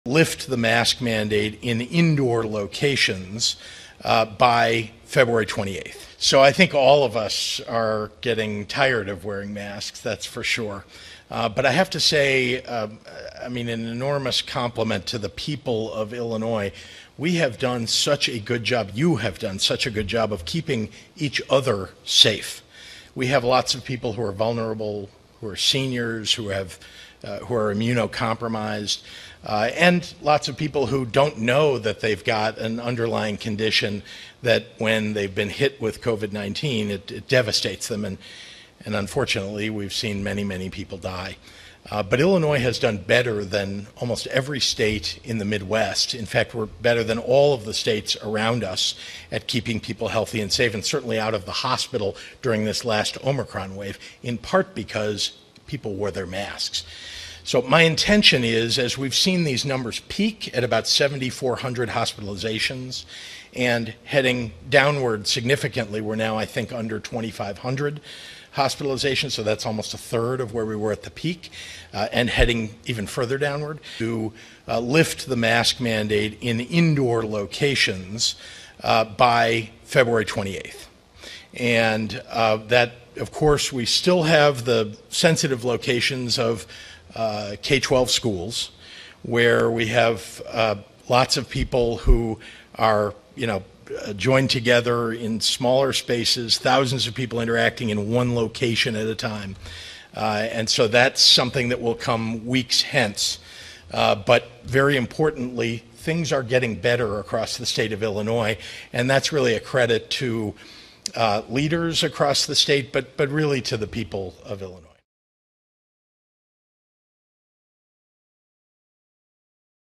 Gov. Pritzker Announces Planned End Date for Illinois Mask Mandate
Speaking during an unrelated press conference, the governor revealed he aims to lift the mandate for the state, with certain exceptions, by Feb. 28